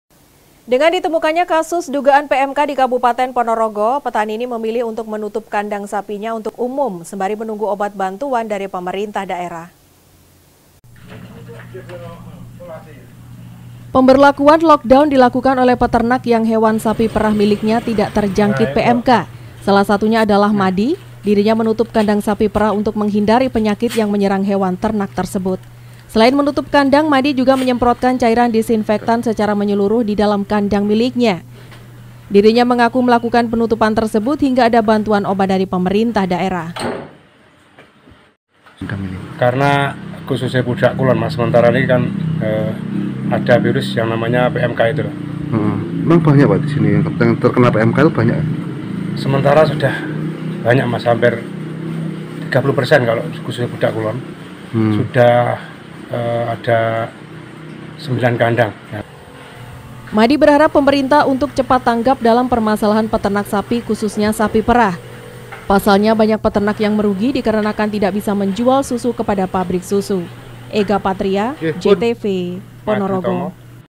PONOROGO - Was Was,Peternak Ini Pilih Lokdown Kandang Sapi
Dengan ditemukanya kasus dugaan pmk di kabupaten ponorogo. Petani ini memilih untuk menutup kandang sapinya untuk umum sembari menunggu obat bantuan dari pemerintah daerah.